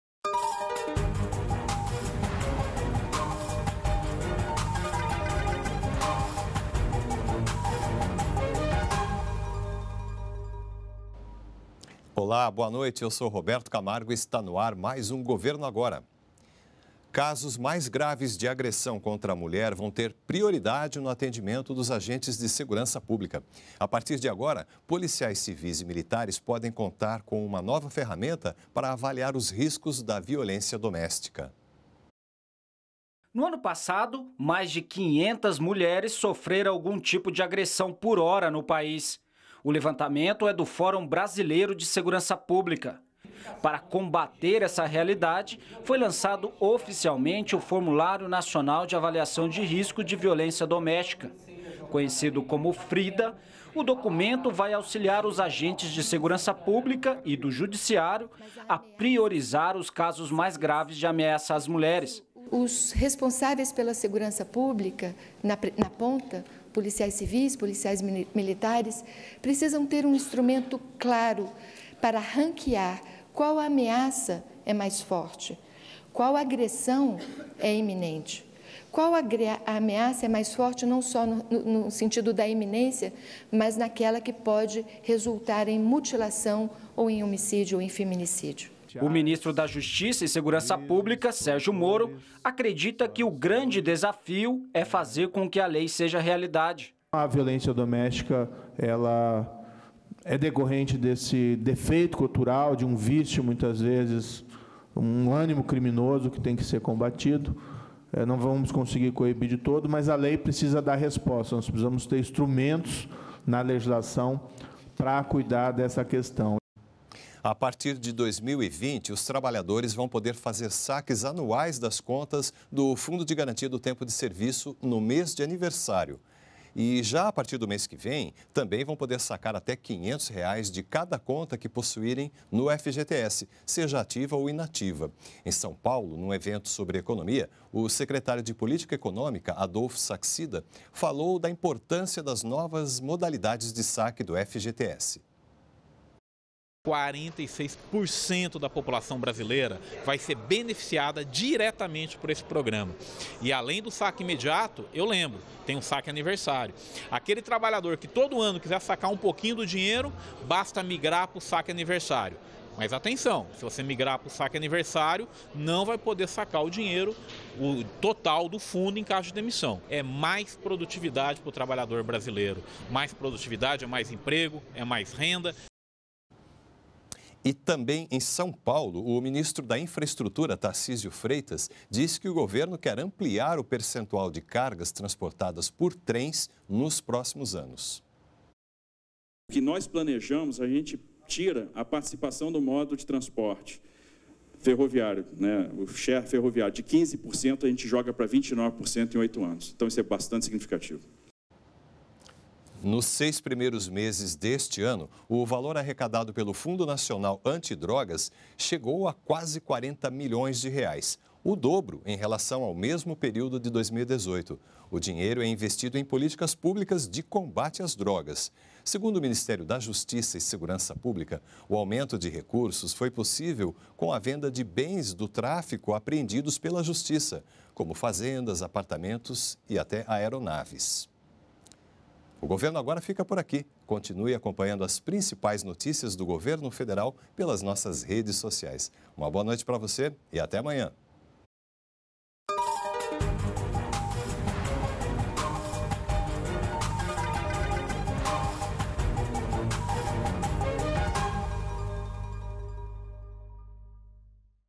Casos graves de violência contra a mulher vão ter prioridade no atendimento dos agentes de segurança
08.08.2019 - Casos mais graves de agressão contra a mulher vão ter prioridade no atendimento dos agentes de segurança pública. A partir de 2020 os trabalhadores vão poder fazer saques anuais das contas do FGTS, no mês de aniversário, e a partir do mês que vem vão poder sacar R$500,00 de cada conta, ativa ou inativa, que possuírem. E também em São Paulo, o Ministro da Infraestrutura, Tarcísio Freitas diz que o governo quer ampliar o percentual de cargas transportadas por trens. E nos seis primeiros meses do ano, o dinheiro arrecadado pelo Fundo Nacional Anti-Drogas foi de quase R$40 milhões.